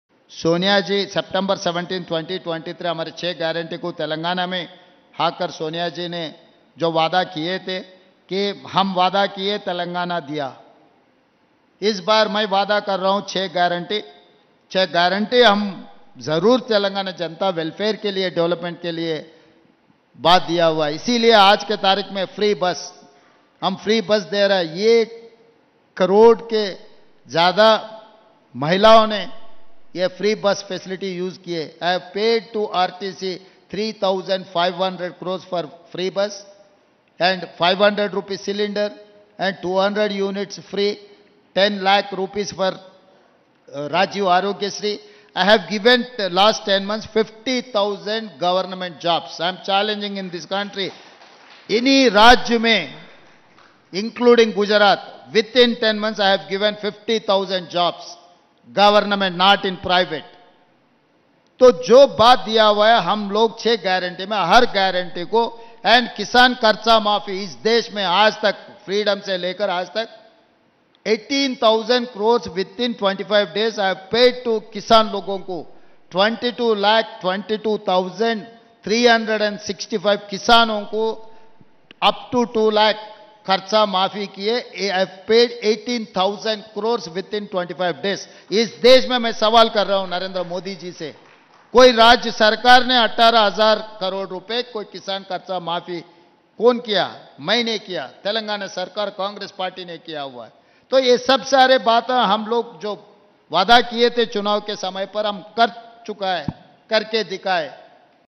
तेलंगाना की कांग्रेस सरकार ने निभाया अपना वादा... | Congress | Revanth Reddy
तेलंगाना में कांग्रेस सरकार ने वादा निभाया 👇

• महिलाओं को फ्री बस सुविधा मिल रही है
• 500 रुपए में गैस सिलेंडर मिल रहा है
• 200 यूनिट मुफ्त बिजली मिल रही है
• राज्य में किसानों का 2 लाख तक का कर्ज माफ किया गया है
• राजीव आरोग्यश्री योजना के तहत 10 लाख तक का इलाज मुफ्त है
• 10 महीने के अंदर सरकार ने 50 हजार सरकारी नौकरियां दी हैं

: तेलंगाना के मुख्यमंत्री श्री @revanth_anumula